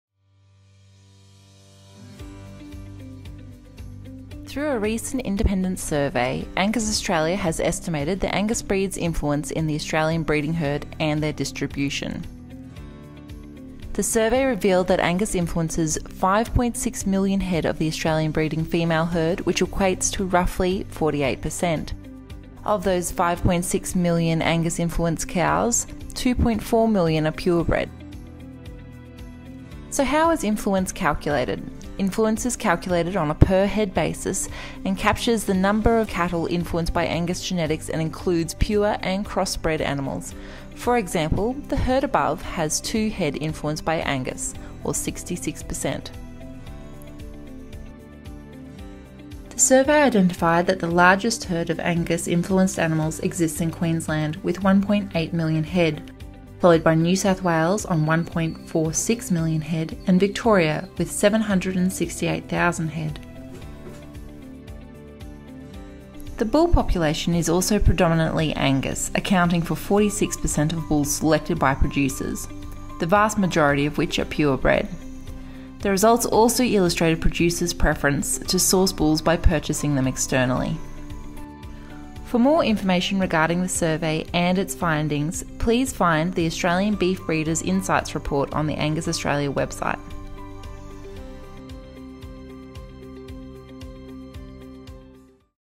Australian Beef Breeding Insights - Angus influences 48% of breeding females
An independent survey has identified that Angus genetics have a significant impact in the Australian cattle population, with substantial penetrance into northern Australia.  The results identified that approximately 5.6 million breeding females in the national beef herd are influenced by Angus genetics, accounting for 48% of the beef breeding population.
The survey, conducted by an independent market research group, as part of a MLA Donor Company supported adoption project, was primarily focused on collecting baseline data on producer’s breeding preferences and genetic technology adoption. In addition, data captured from the survey was used to quantify the level of Angus penetration in the Australian beef industry.

This video was created as a result of a collaboration between Angus Australia and Meat & Livestock Australia Donor Company (MDC) (Project P.PSH.1063).